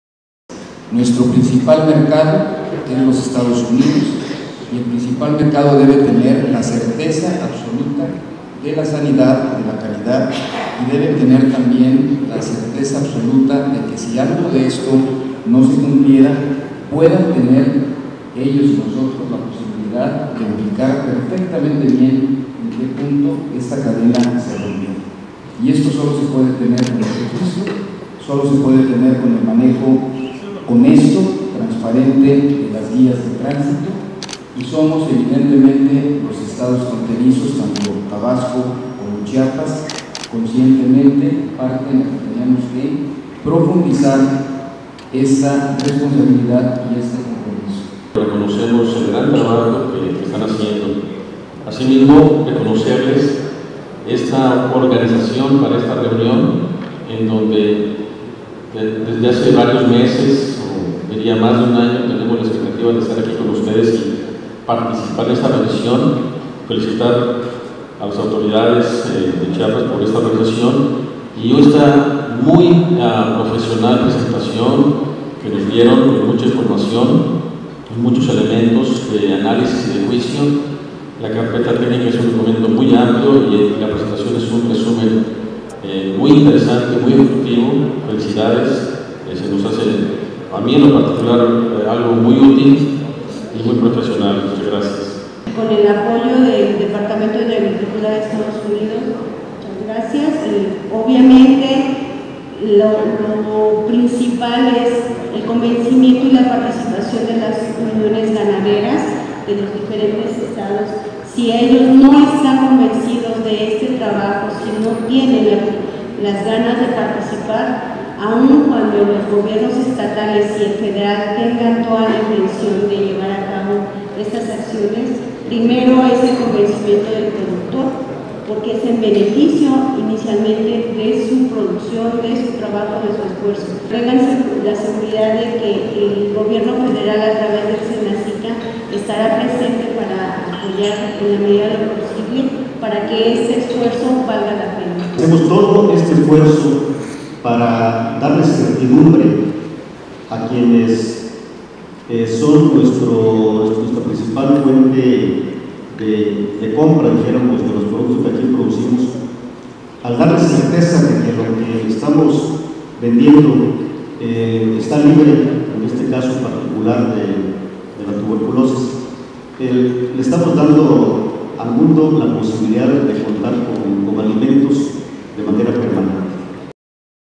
EVENTO SENASICA
SAGARPA, SENASICA y USDA APHIS, inician revisión para precertificación de Baja Prevalencia de Tuberculosis Bovina de Chiapas y la Región Sureste Istmo de México.